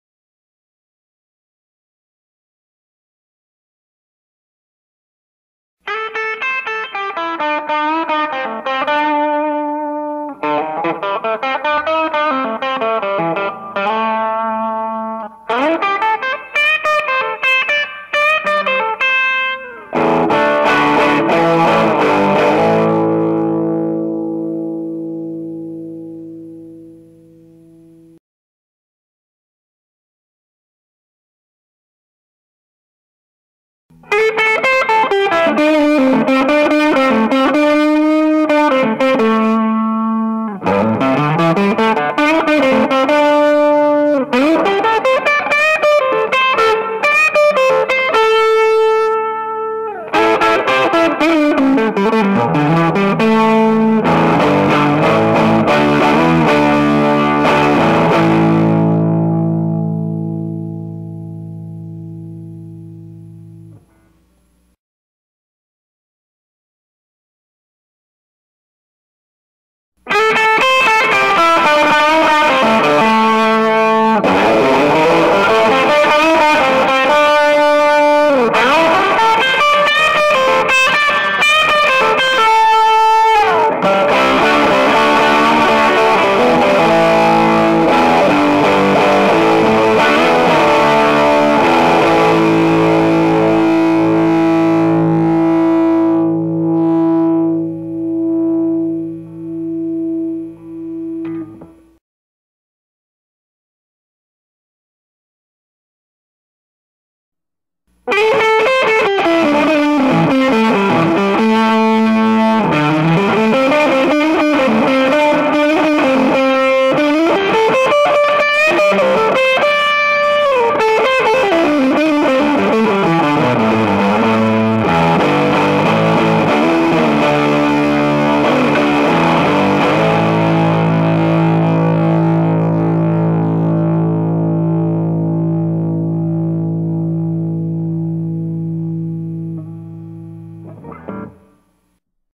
Les Paul for sale
1994 Korean made Gibson/Epiphone Les Paul sound demo. This guitar is played through a Egnater Tweaker amp.